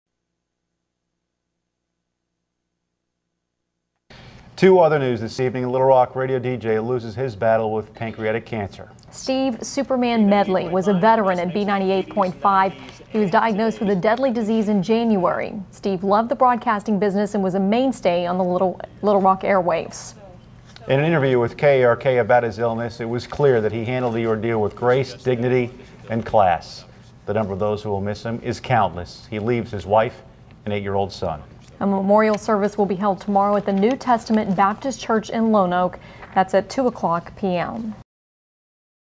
Superman Steve Medley's Battle with Cancer is over
Though we gather here today, bound together in sorrow and loss, we share a precious gift. We are, all of us, privileged to live a life that has been touched by Superman Steve Medley. The Man of Steel possessed many extraordinary gifts, and he shared them with us freely. None of these gifts were more remarkable than his ability to discern what needed to be done, and his unfailing courage in doing it, whatever the personal cost. Let us all strive to accept his gift, and pass it along, as an ongoing tribute to Steve, who taught us all how to be heroes.

Goodbye Brother